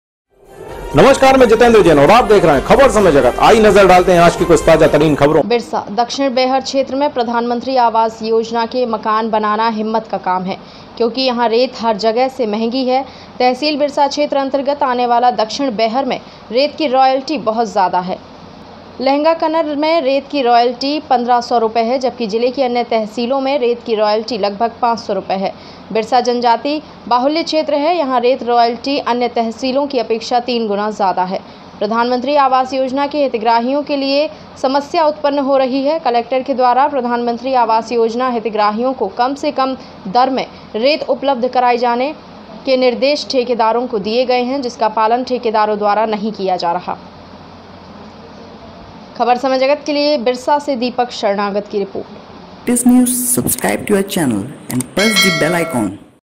बिरसा - महंगी रेत के कारण पीएम आवास बनाना हुआ मुश्किल
बिरसा से दीपक शरणागत की रिपोर्ट